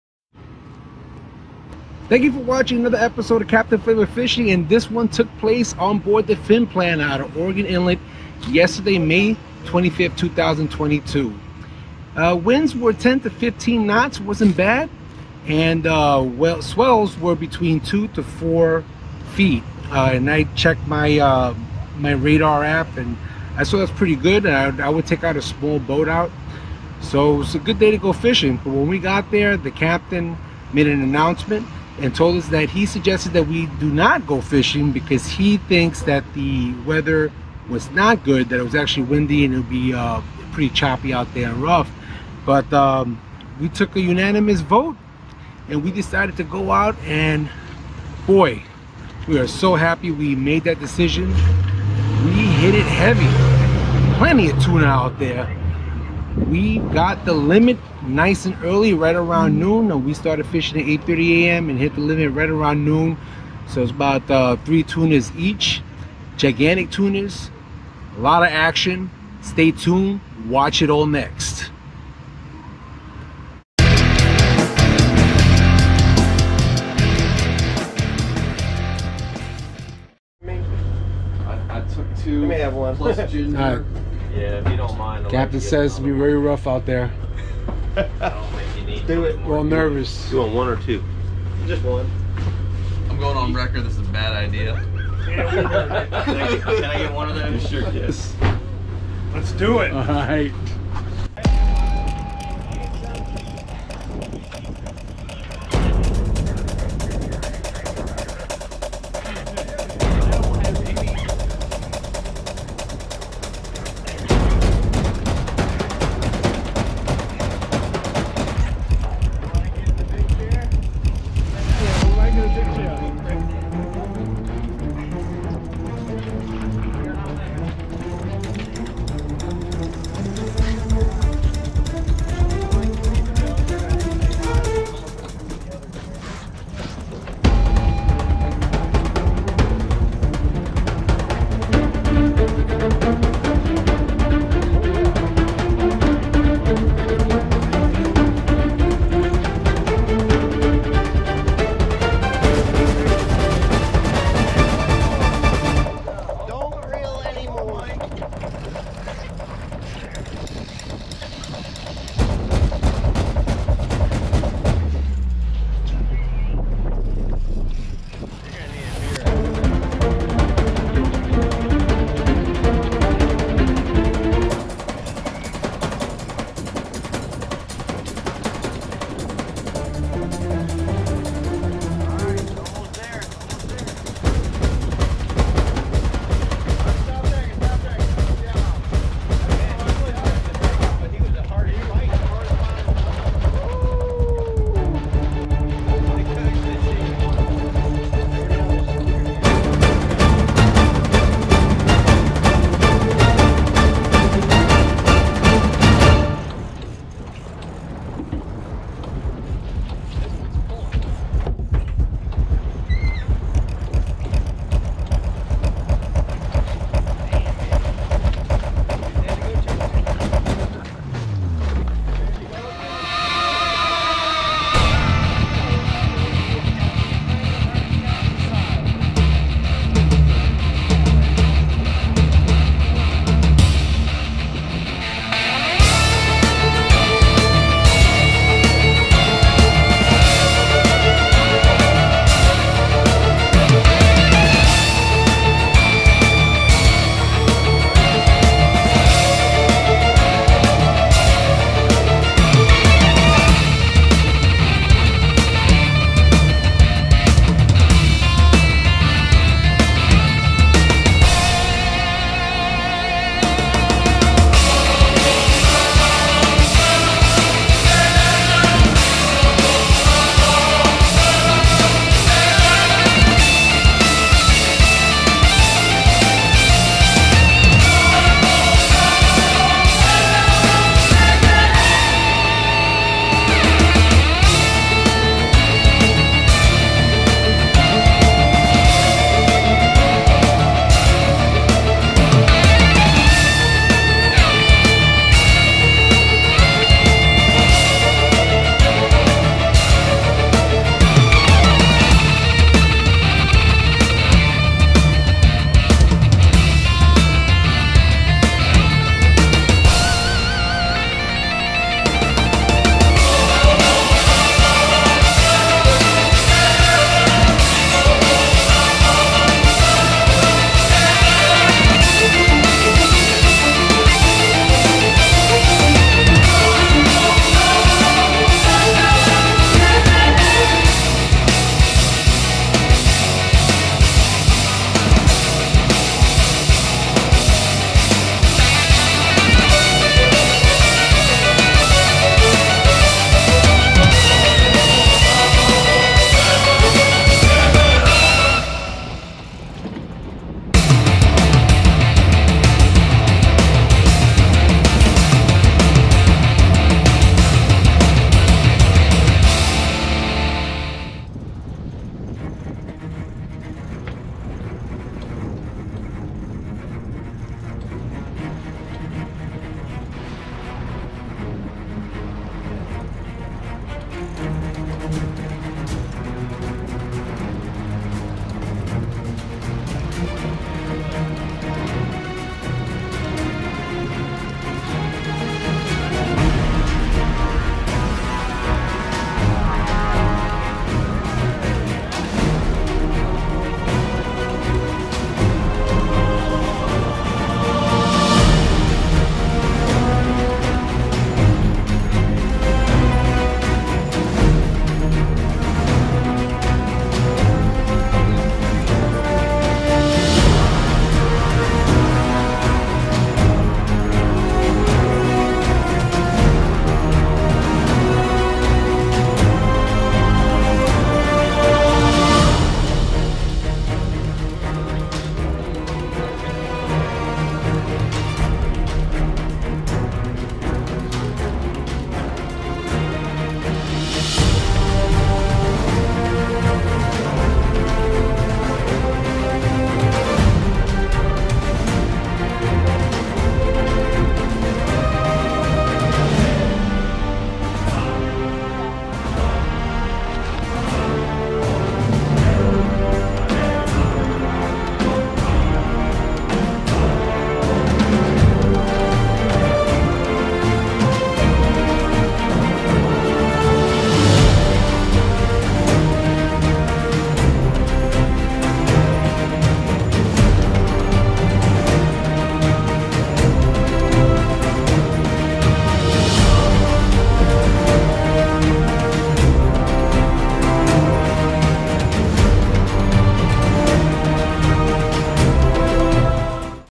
Fin Planner Sports Fishing Oregon Inlet 2022
Overall great experience on 5/25/22. The cabin was clean well kept. The galley had a Frig and microwave with split sonar screen of what the Captain was watching. The Captain did not recommend going out because of weather but it didn't match up with the local marine forecast. On that day winds was 10-15 knots and swells 2-4 feet. I usually go out fishing with a 16 foot boat on those conditions. We voted to still go out and killed the TUNA! Out on fishing waters there were smaller crafts around 21-26 foot with single outboards fishing.